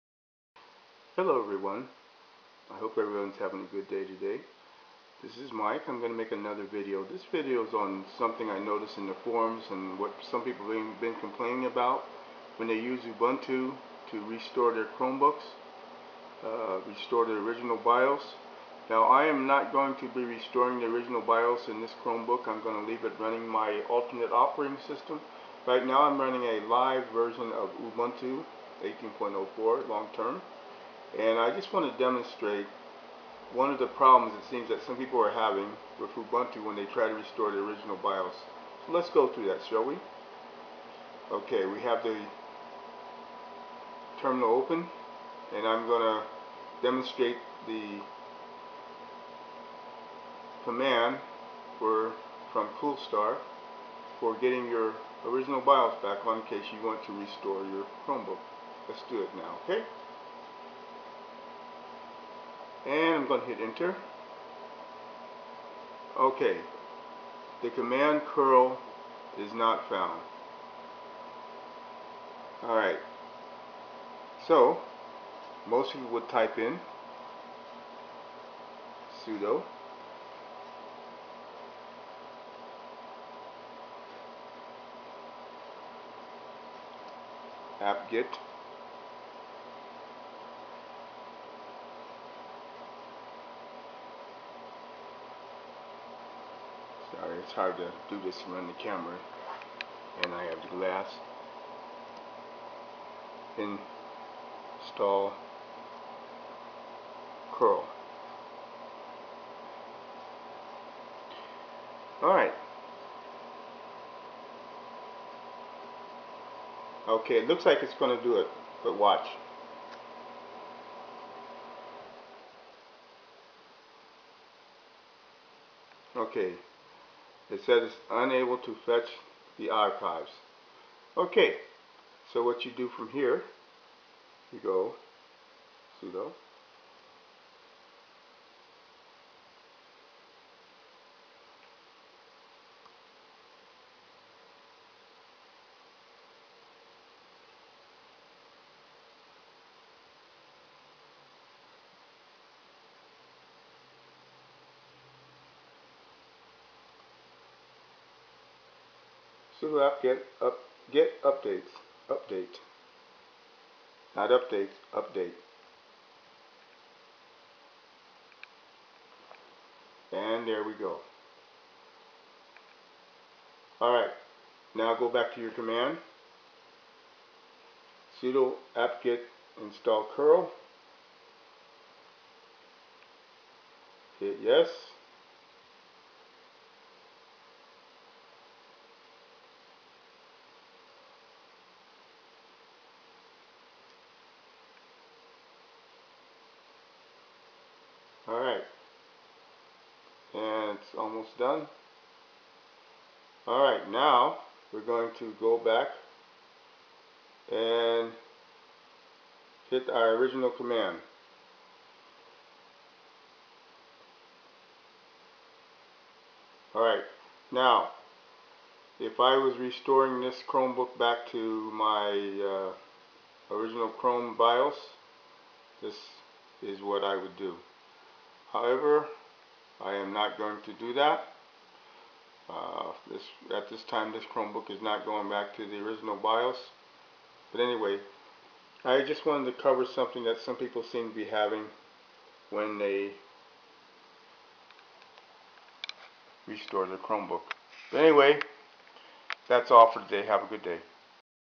Trouble restoring your Chromebook bios? try this.
Many people use linux to restore their original bios. I am showing one sticking point for some.